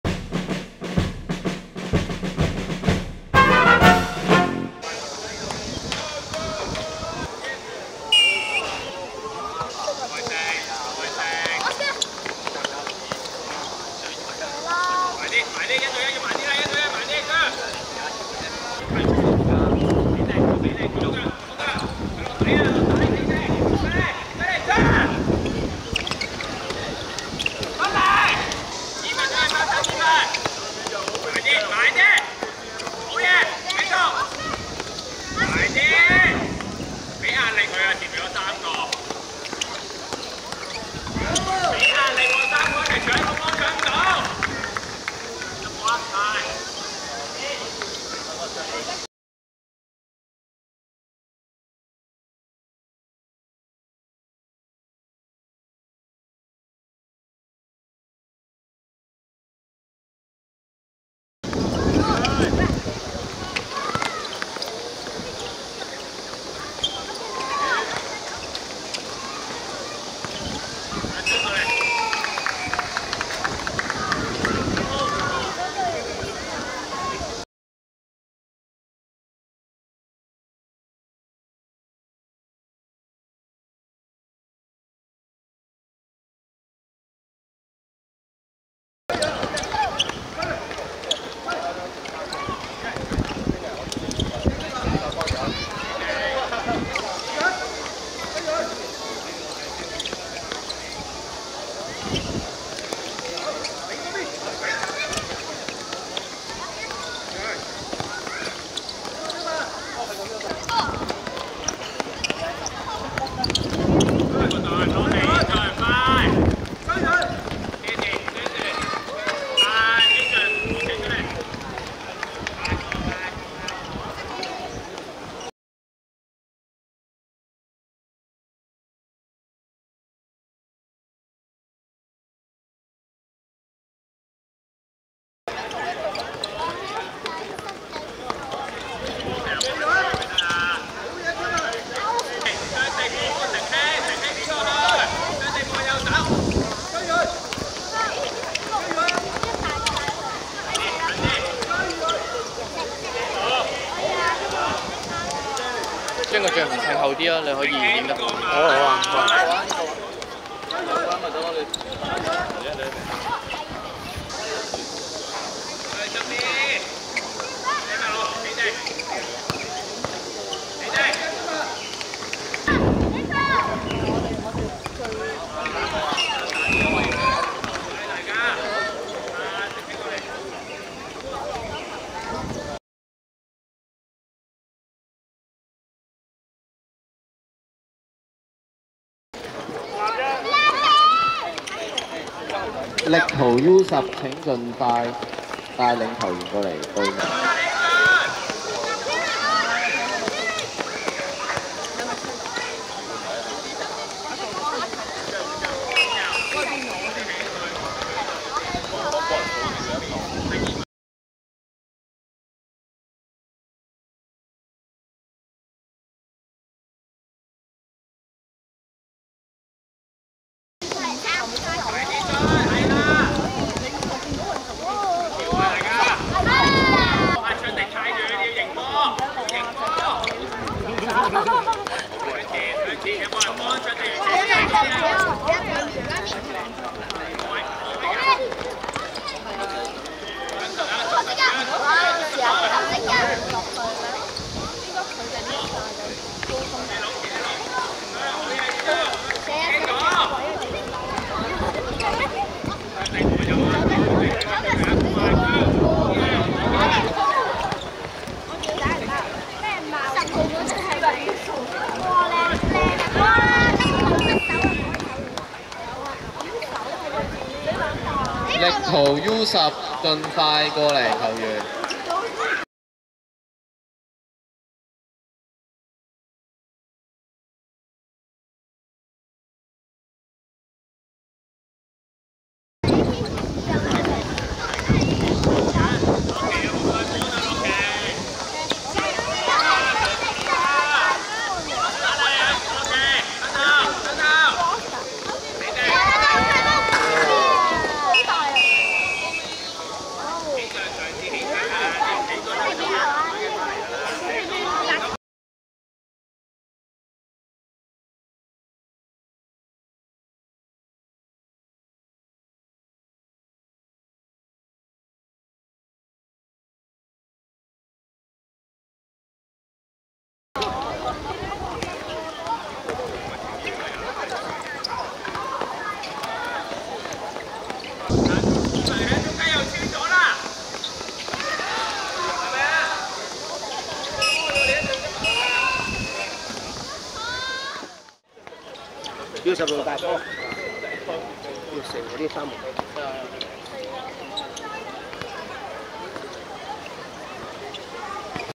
A.F.Soccer vs Boas (2024.6.30 We Sports 回歸盃5人足球賽~U10分組賽)精華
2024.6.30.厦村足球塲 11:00
A.F.Soccer(藍) 0 : 7 Boas(綠)